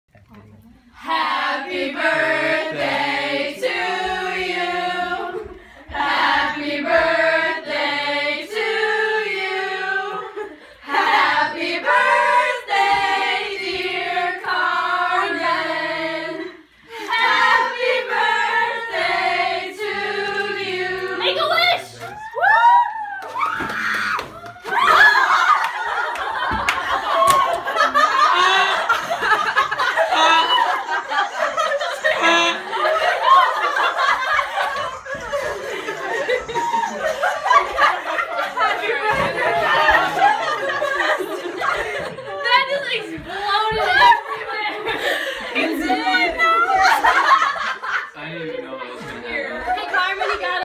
Birthday Suprise
Carmen gets a special treat on a special day.